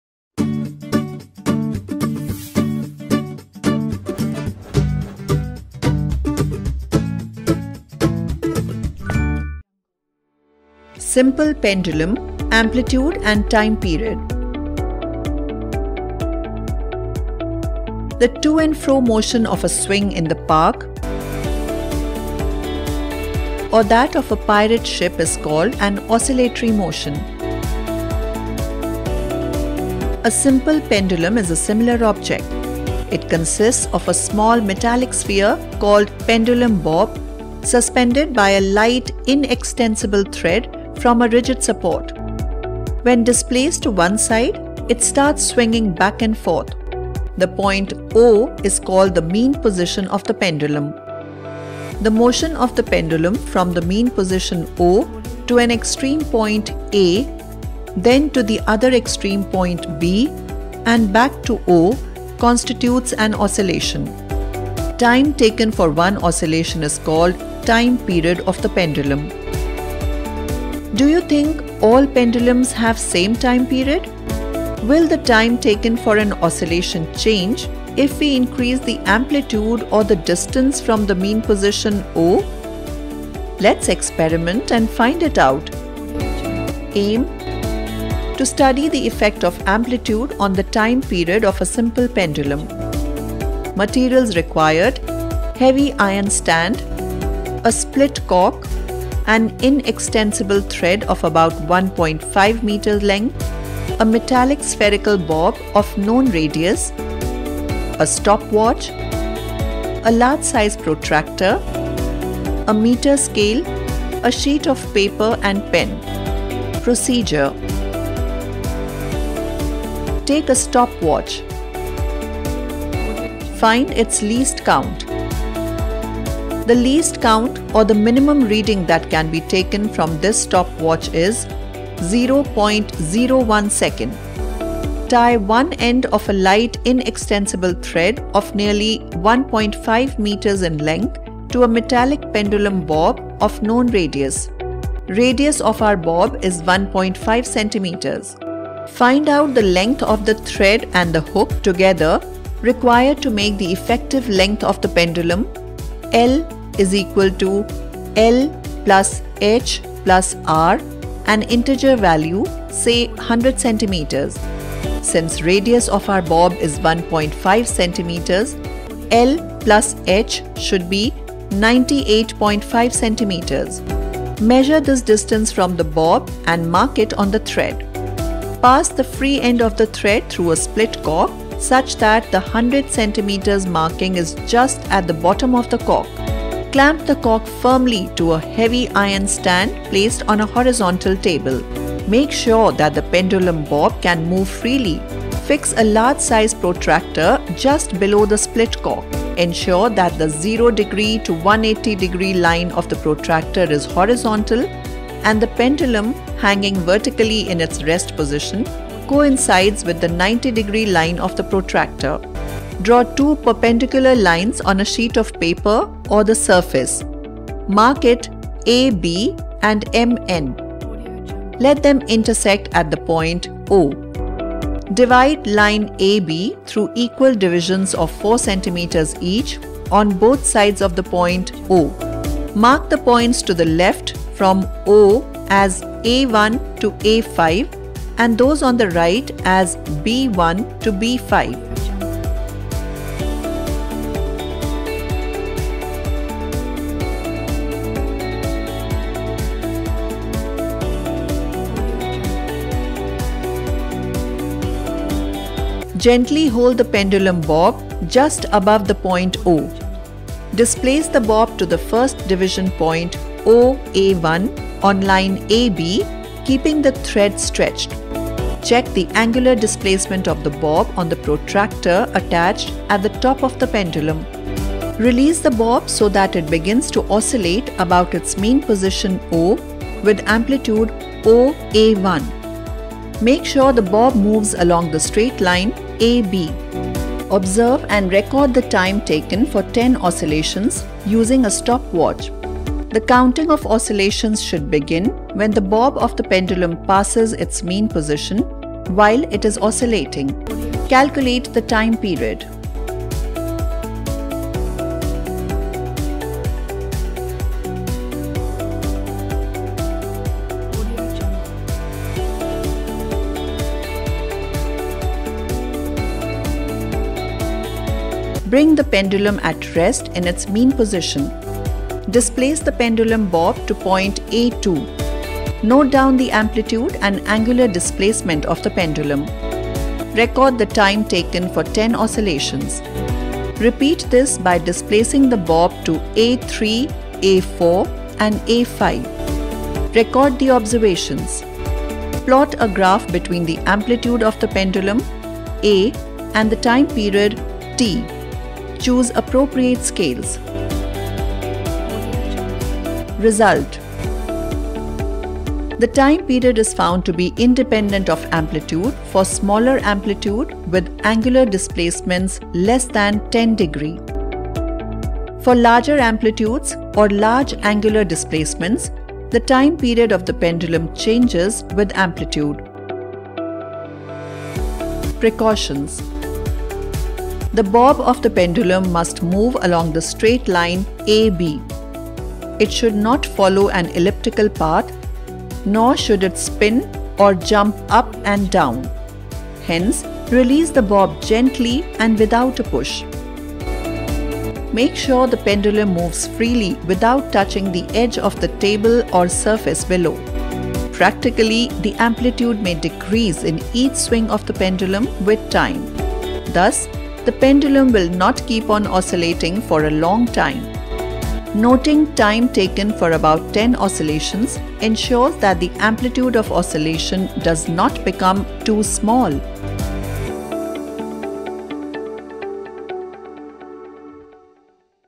Simple pendulum
A simple pendulum consists of a small object made of a heavy metallic sphere, called the pendulum bob, suspended by a light inextensible thread from a fixed and rigid support. When displaced to one side and released, it begins to oscillate about its mean position. The time taken by the pendulum to complete one oscillation is called its time period. The maximum displacement of the pendulum from its mean position is called its amplitude.

The experiment in this video aims to investigate the effect of amplitude on the time period of a simple pendulum.